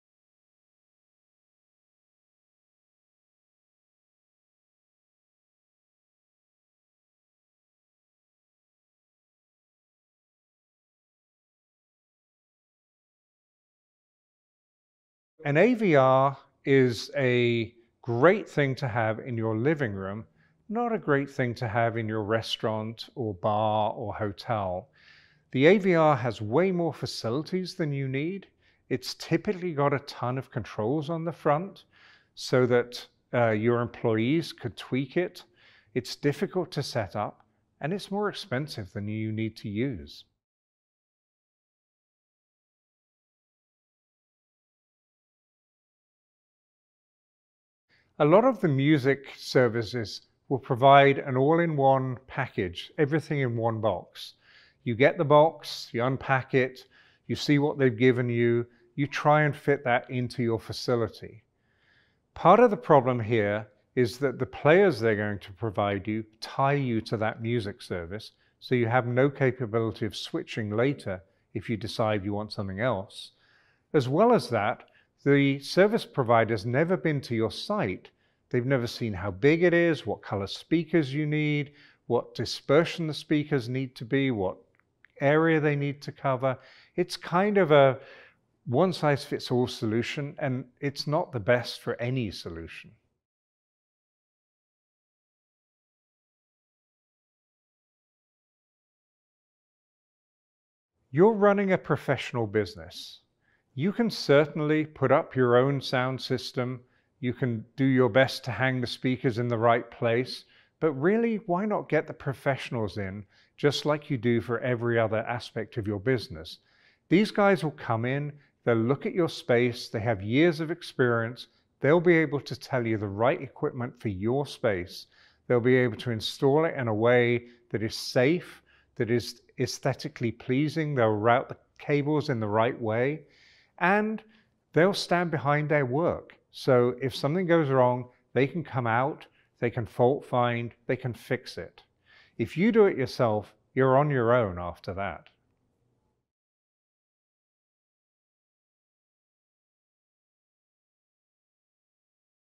Bluesound Professional - FAQs from Commercial Integrators
Why use a Bluesound Professional player over having separate players and amps in a rack? Should you get the B170S when you already have the B160S? Why is 70/100V important? Get answers to commonly asked questions about Bluesound Professional products.

Learn more about Bluesound Pro at bluesoundprofessional.com.